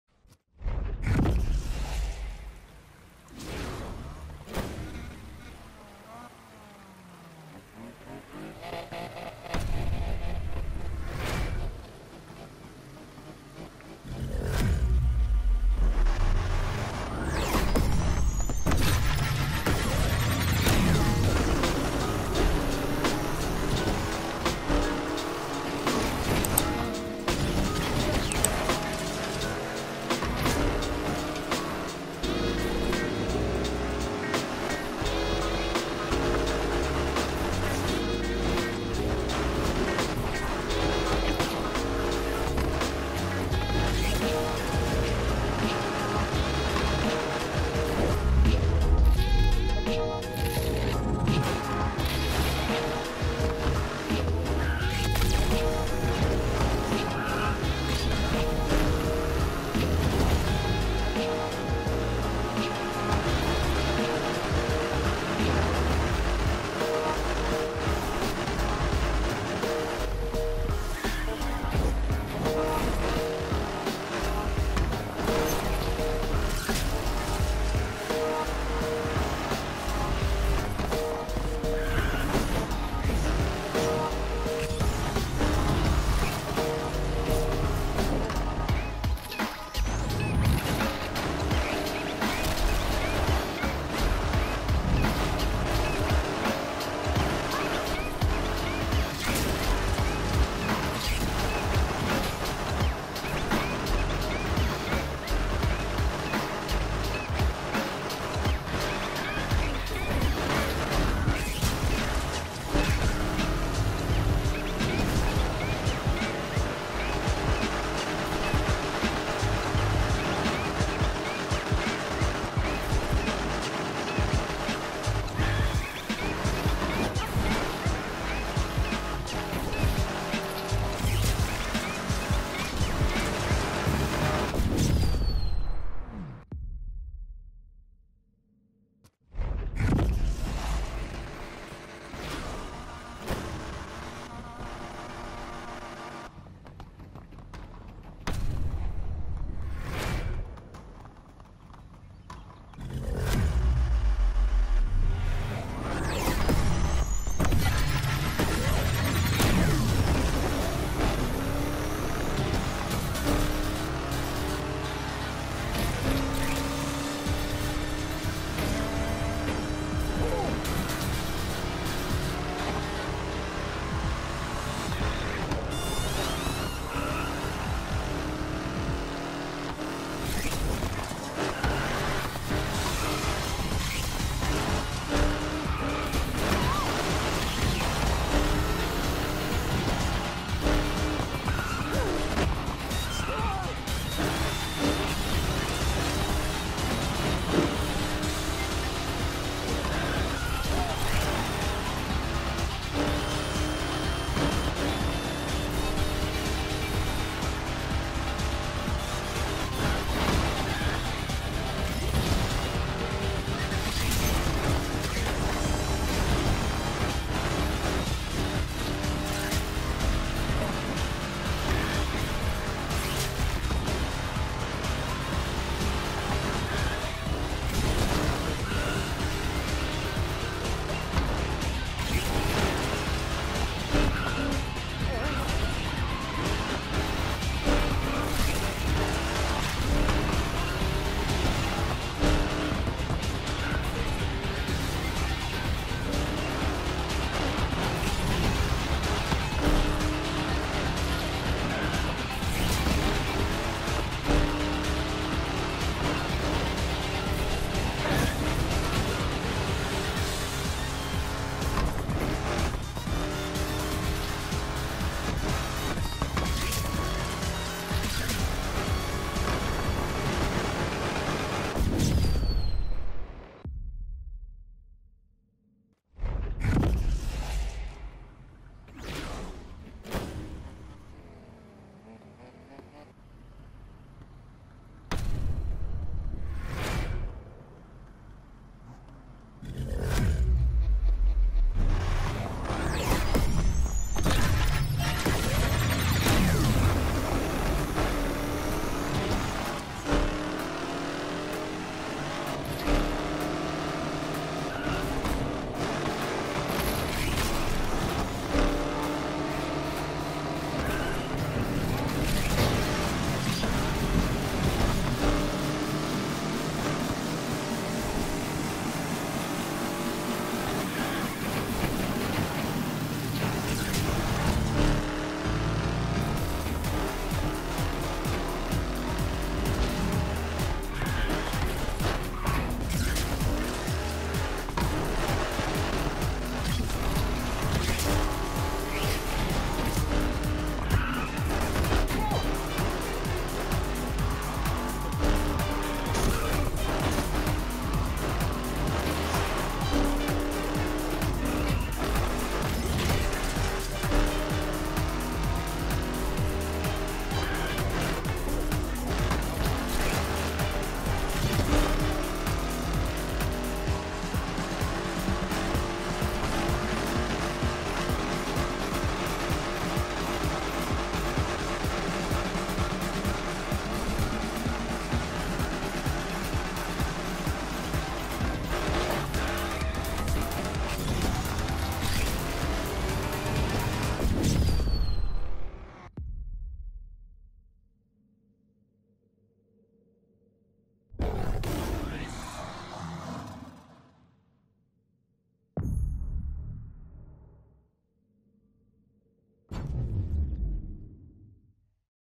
Toe-to-toe against a Carrera RSR
B Tier Playlist. Some very fast rivals compete, and I race in my modified Testarossa. One rival in their Porsche Carrera RSR is very formidable.